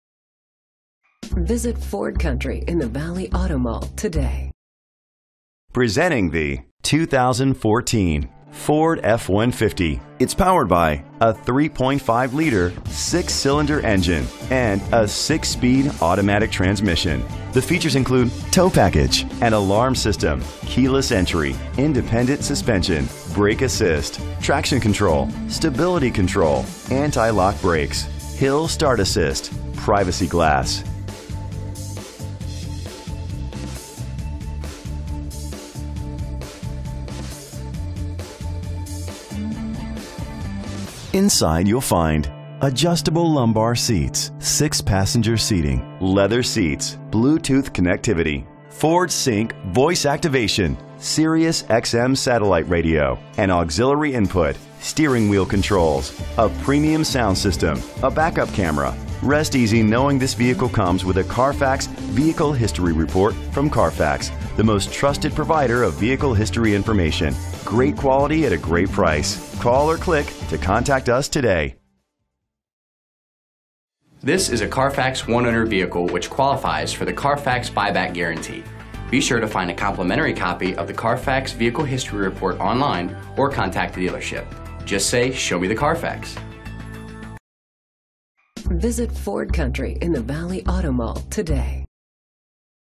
2014 Ford F-150 52506A - Henderson NV
Year: 2014
Make: Ford
Model: F-150
Trim: XLT
Engine: 3.5 liter 6 Cylinder EcoBoostcylinder 2WD
Transmission: 6-Speed Automatic
Color:
Mileage: 35377
Address:
VIN:1FTFW1CTXEKD06963